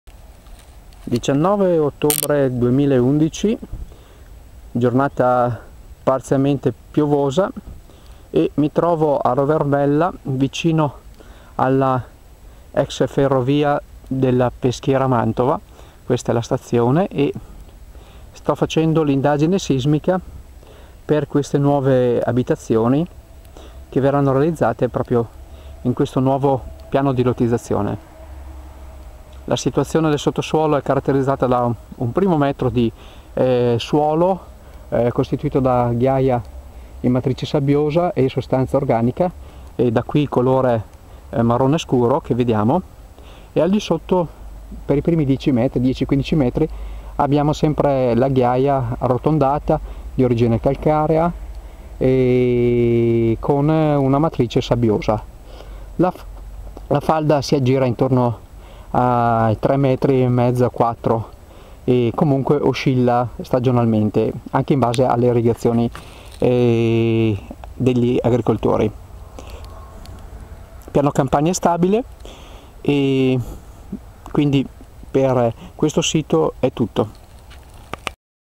111019_11100_Indagine sismica a Roverbella.AVI
(11100)  Indagine sismica a Roberbella il 19 ottobre 2011 by Marco Melli.